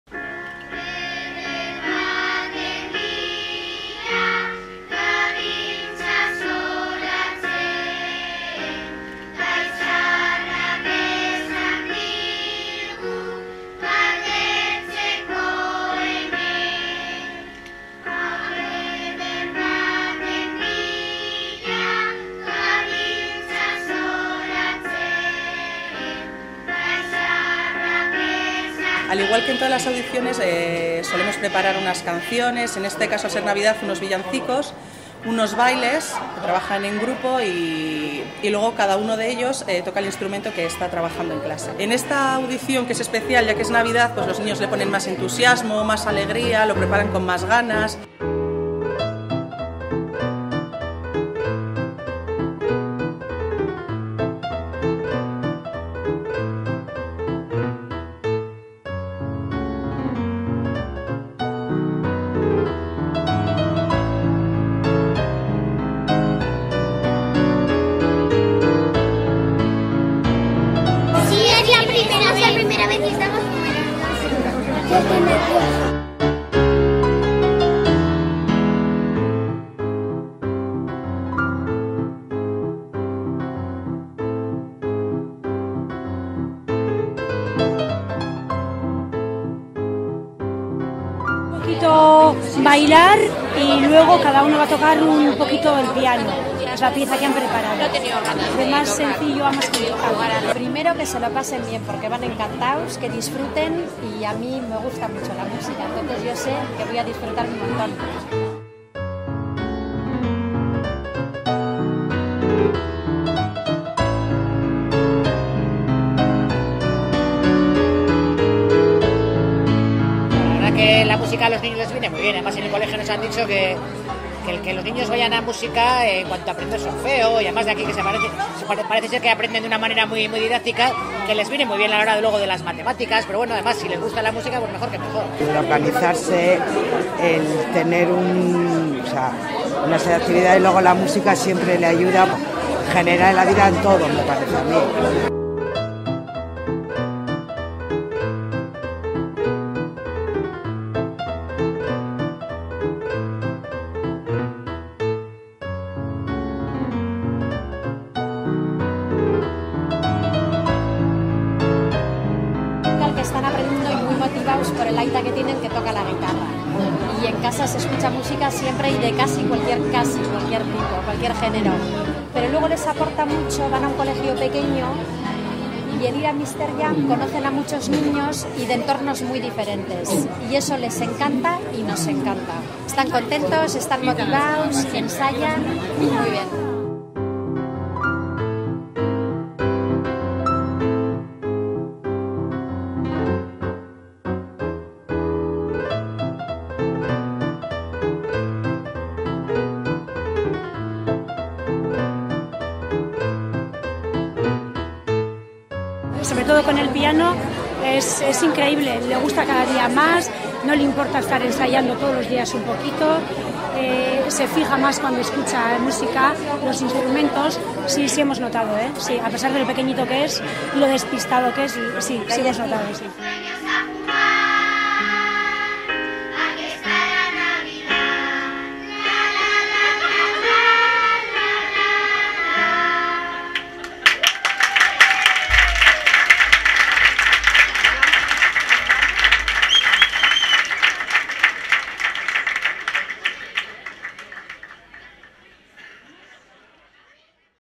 Fiesta Infantil de Navidad de MrJam 2014
El 12 de diciembre de 2014 celebramos en la FNAC de Bilbao la Fiesta Infantil de Navidad. En ella los alumnos más pequeños de MrJam CMM enseñaron sobre el escenario todo lo que habían aprendido en clase durante los últimos meses, y de paso, nos felicitaron las Navidades con algunos villancicos.